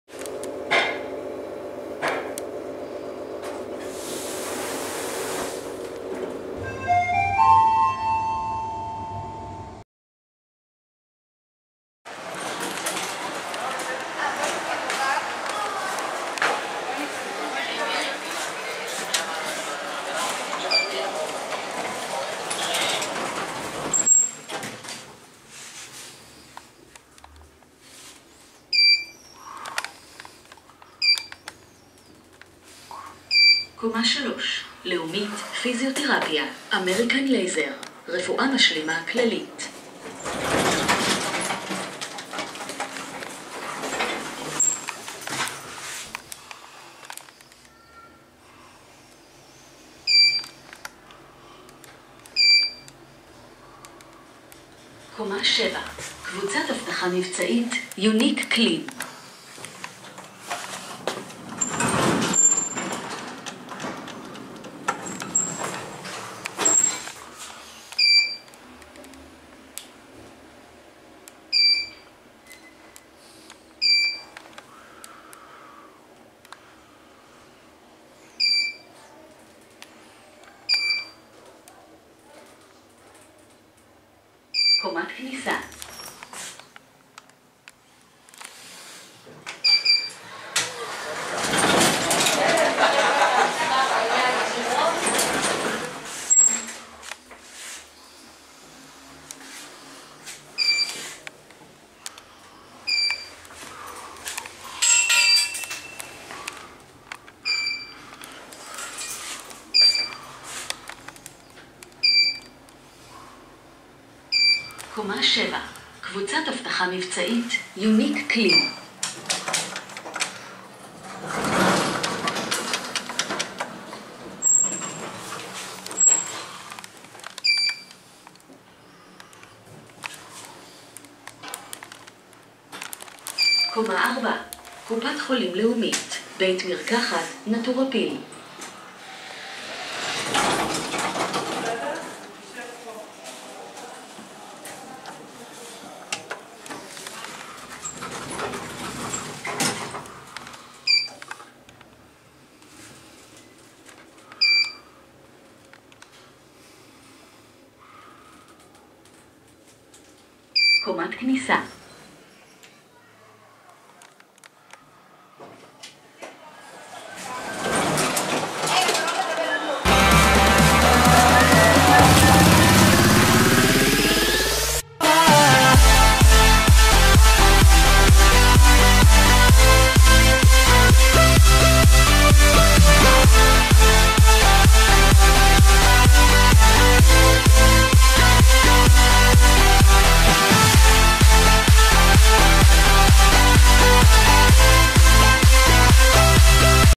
Ofer Rehovot Mall, Rehovot, IL | 1995 Electra (mod.) traction lifts (Offices A).
---Office elevators A---
Brand: Electra
Moderniser: Electra
History: Modernised
Year: 1995
Modernised: 2010's
Fixtures: OTIS CONCAVE
Capacity:
Type: Traction
Floors: 8(*L*, 1, 2, 3, 4, 5, 6, 7)
Elevator bank size: 2

Reusing of this video is forbidden without permission.